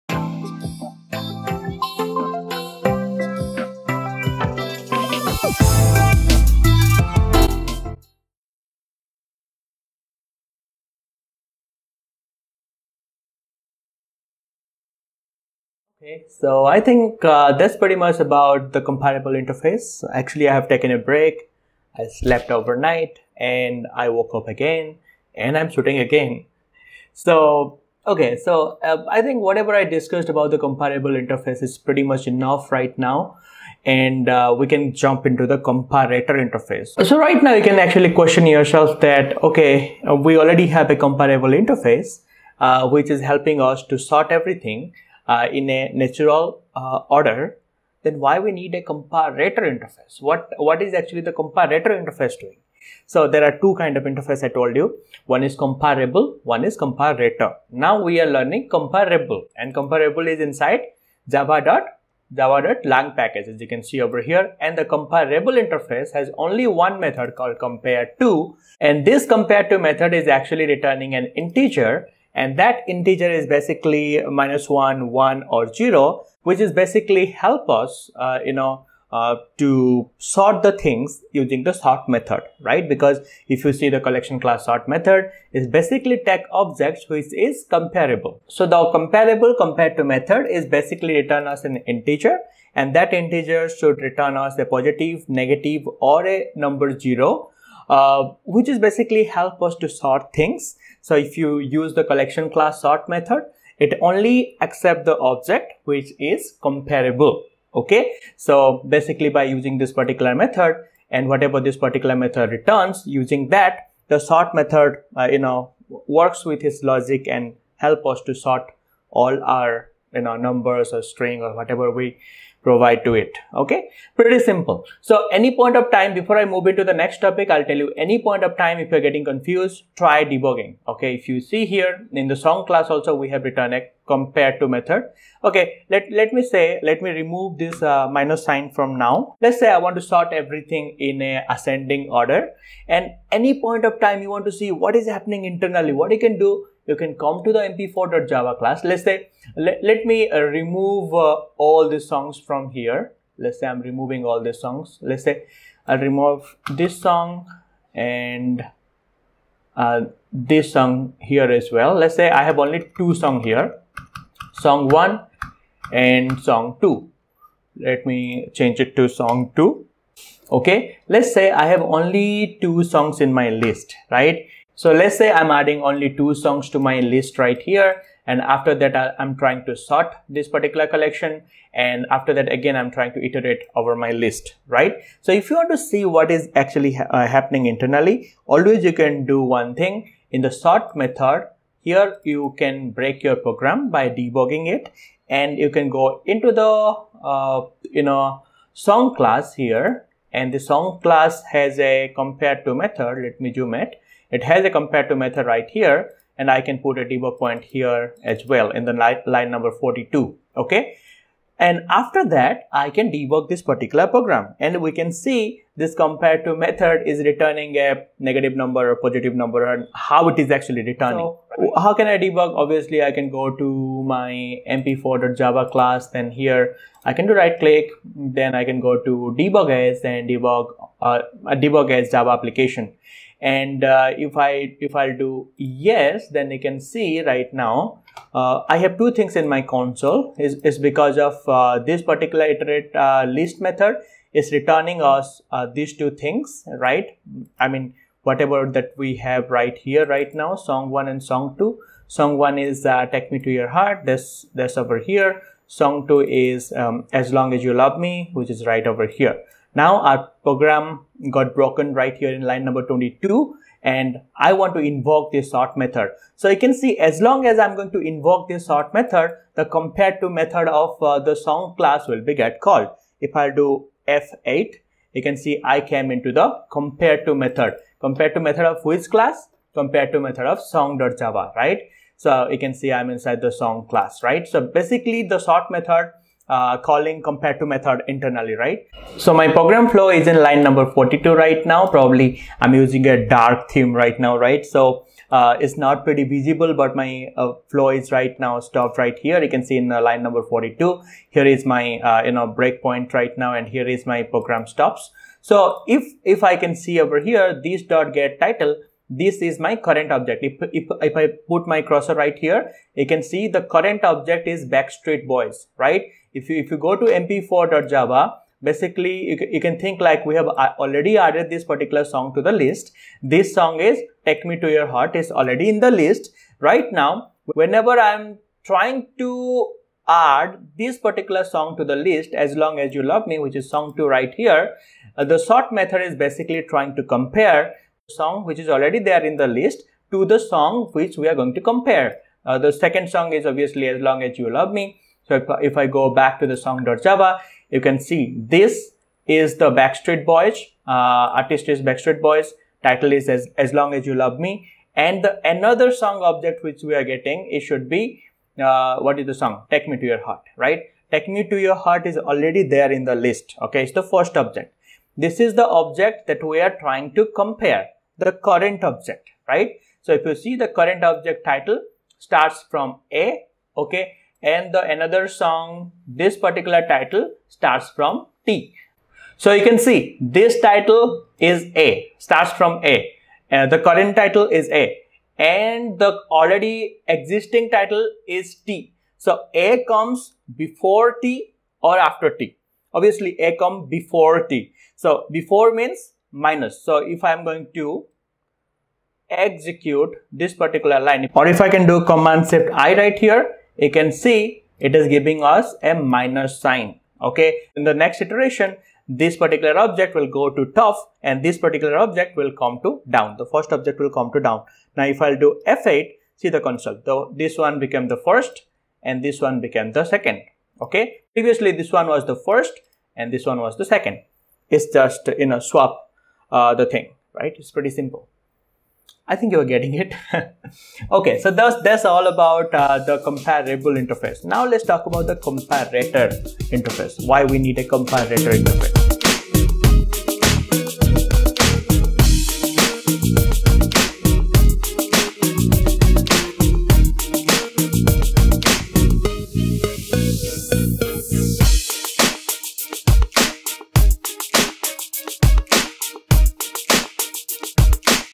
Comprable : behind the scene [debugging] | Java Collection Framework | Collection.sort | #6
In this video, we will wrap up the comparable interface in java. We will debug our current program to understand the flow and to confirm the behind the scene kinds of stuff. So these are a few things we understood so far in this course.

1)Comparable is an interface
2)It has a method called comapreTo() which helps to sort.
3)Comparable interface present in java.lang package.
4)The comparable interface helps in natural sorting
5)All wrapper class and String class are by default implement comparable and already.